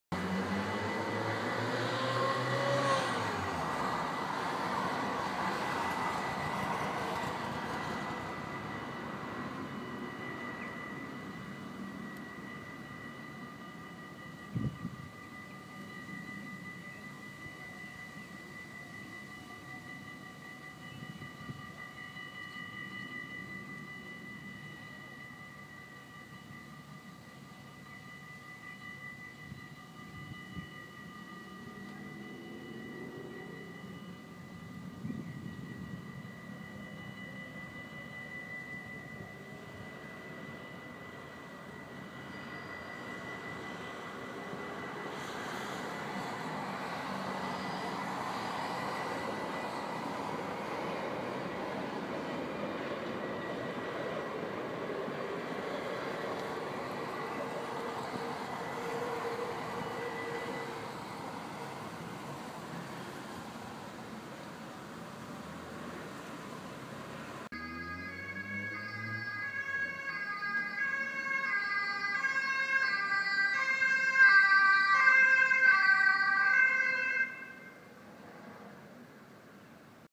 A2/A1 ambulance 16157 + 09132 naar ongeval met fiets bodegraven 14-06-2014
17:03:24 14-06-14 -ALPHA- A2 Schoutendreef 42 thv BDN 2411GC : Vtg: 16157 Ritnr: 60284
17:13:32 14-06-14 -ALPHA- A1 Schoutendreef 42 thv 2411GC 42 thv Bodegraven
 0726132 Ambu 09-132 (RAVU)
17:13:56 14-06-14 -ALPHA- Prio 1 Schoutendreef 42 thv BDN Wegvervoer ongeval letsel (fiets)